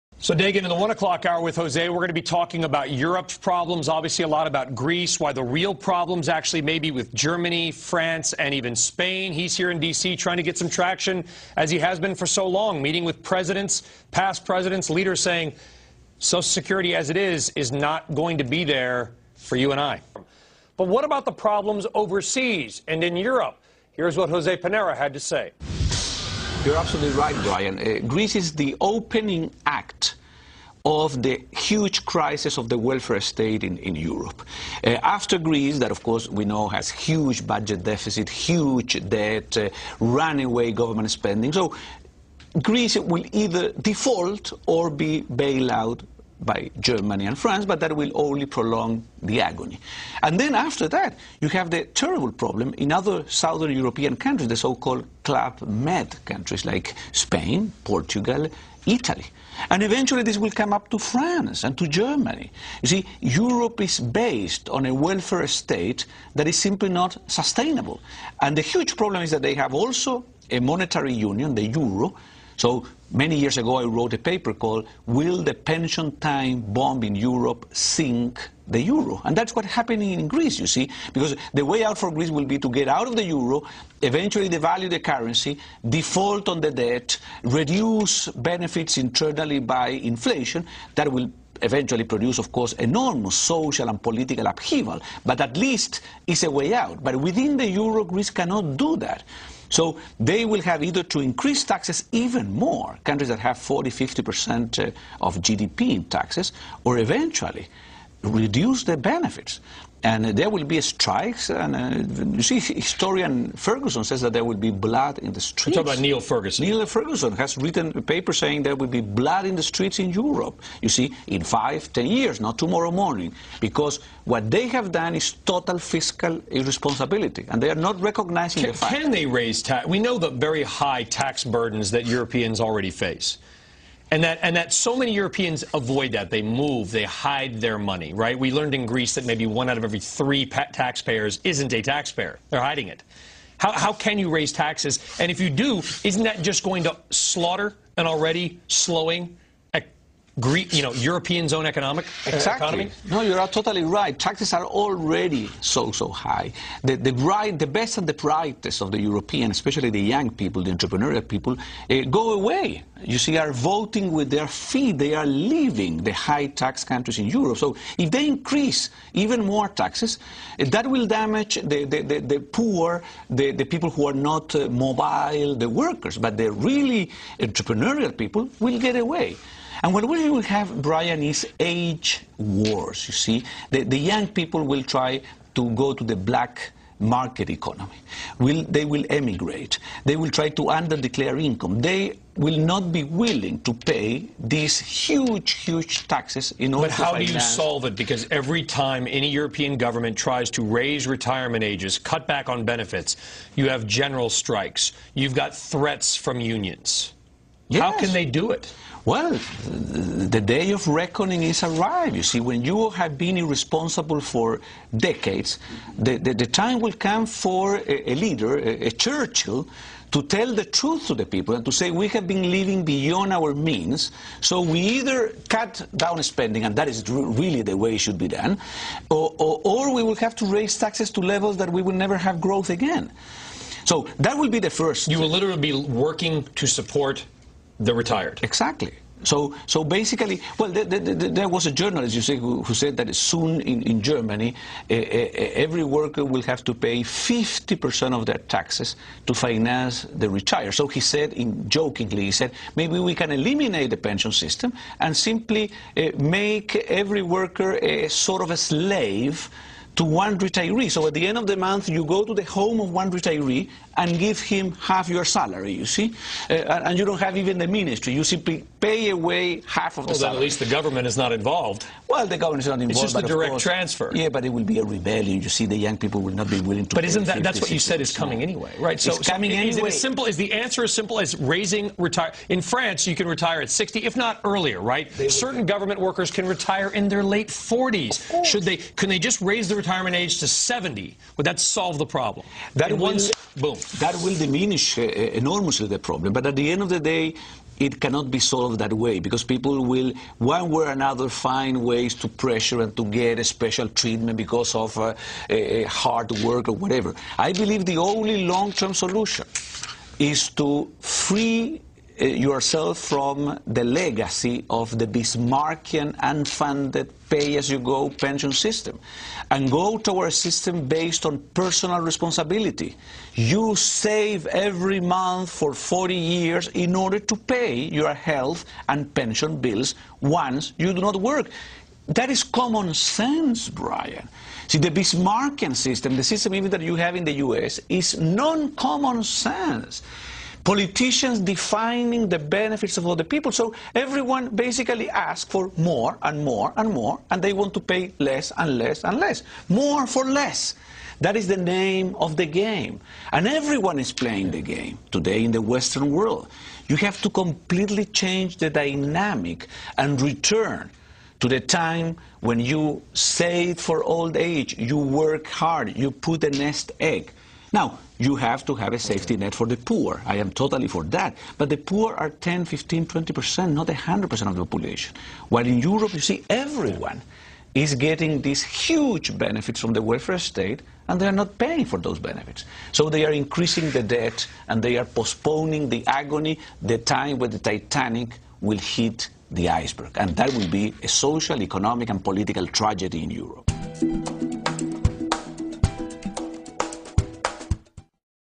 José Piñera: The coming European crisis
Fox Business News interview to José Piñera
President, International Center for Pension Reform
Distinguished Senior Fellow, Cato Institute